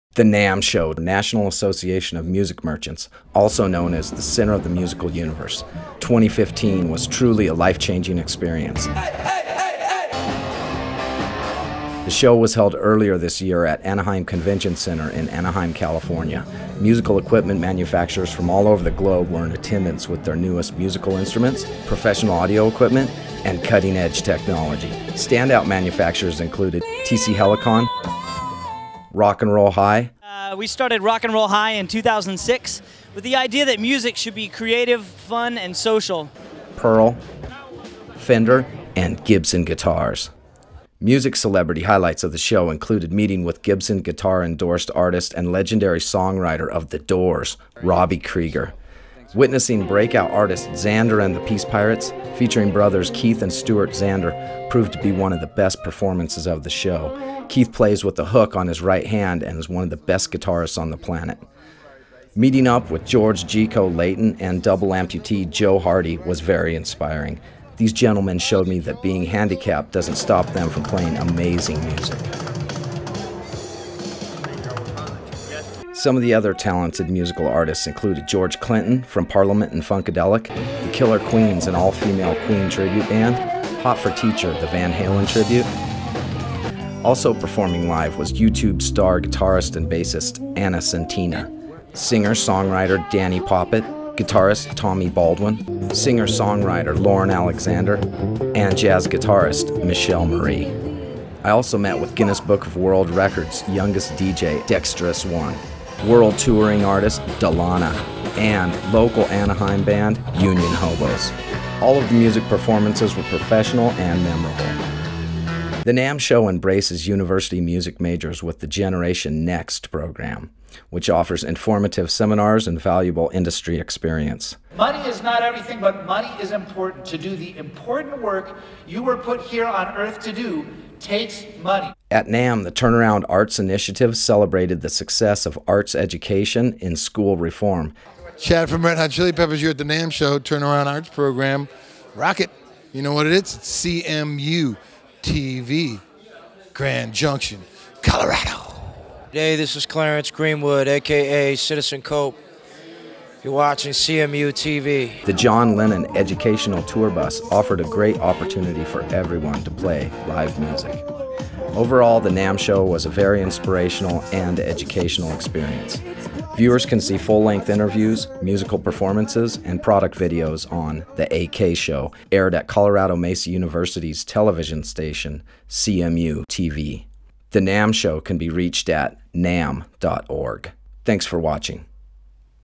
NAMM 2024 Robby Krieger, Chad Smith, Citizen Cope, Anna Sentina, Dani Mathers & more!
Special thanks goes out to NAMM, Playboy Playmate Miss May 2014 Dani Mathers, Clayton guitars, Whitney Myer from TC Helicon, Aaron Burch Rock and Roll High, Pearl Drums, Fender Guitars, Gibson Guitars, music celebrities Robby Krieger of The Doors, guitarist Charlie Parra del Riego, Keith Xander and brother Stuart Xander from Xander & the Peace Pirates, George Gko Layton, Joe Hardy, George Clinton from Parliament and Funkadelic, Nina Noir from The Killer Queen’s, Hot for Teacher, YouTube star guitarist and bassist Anna Sentina, singer songwriter Dani Poppitt, guitarist Tommy Baldwin, singer songwriter Lauren Alexander, jazz guitarist Michelle Marie, Guinness book of world records youngest DJ Dexterous One, world touring artist Dilana, Gus Graf and Steve Smith from local Anaheim band Union Hobos, Generation Next Program, Turnaround Arts Initiative, Chad Smith from Red Hot Chili Peppers, Clarence Greenwood a.k.a. Citizen Cope, and John Lennon educational tour bus.

NAMM overview video 2015 is a glimpse into the National Association of Music Merchants tradeshow where the leading musical product manufactures gather every January in Anaheim California to show their newest products. – created by Aaron AK Kennedy

Links to manufactures and guests

Transcription of video voice over by Aaron AK Kennedy –
The NAMM Show 2015 was truly a life-changing experience. The NAMM acronym stands for National Association of Music Merchants also known as the center of the musical universe. The show was held earlier this year at the Anaheim convention Center in Anaheim California. Musical equipment manufacturers from all over the globe were in attendance with their newest musical instruments, professional audio equipment, and cutting-edge technology. Standout manufactures included Personus, TC Helicon, Clayton guitars, Sennheiser microphones, Rock and Roll High, Blackbird, Radar, Dean guitars, DDrum, Pearl, DW, Gretsch guitars, Dialtone Pickups, Fender, and Gibson guitars. Music celebrity highlights of the show included meeting with Gibson Guitars endorsed artist and legendary songwriter of The Doors Robby Krieger. Witnessing breakout artist Xander and the Peace Pirates (featuring brothers Keith and Stewart Xander) proved to be one of the best performances of the show. Keith plays with hook on his right hand and is one of the best guitarists on the planet. Meeting up with George Gko Layton and double amputee drummer Joe Hardy was very inspiring. These gentlemen showed me that being handicap doesn't stop them from playing amazing music. Some of the other talented musical artists include George Clinton from Parliament and Funkadelic, The Killer Queens an all female Queen tribute band, Hot For Teacher the Van Halen tribute band, Judas Priestess, YouTube star guitarist and bassist Anna Sentina, singer-songwriter Danni Poppitt, guitarist Tommy Baldwin, singer-songwriter Lauren Alexander, jazz guitarist Michelle Marie, Guinness Book of World Records youngest DJ Dexterous One, World touring artist Dilana and the local Anaheim band Union Hobos. All of the music performances were professional and memorable. The NAAM show embraces's university music majors with their Generation Next program which offers informative seminars and valuable industry experience. Topics included "exploring your potential for a career in the music products industry, The secret to sustainable music careers, and an introduction to intellectual property for today's music professional." The Turnaround Arts Initiative celebrated the success of arts education in school reform and was hosted by Clarence Greenwood a.k.a. Citizen Cope, Tony winner John Lloyd Young, and Chad Smith from the Red Hot Chili Peppers. The John Lennon educational tour bus offered a great opportunity to play live music. Overall The NAMM Show was a very inspirational and educational experience. Viewers can see full length interviews, musical performances, and product videos on The AK Show aired at Colorado Mesa University's television station CMU TV. The NAMM Show can be reached at NAMM.org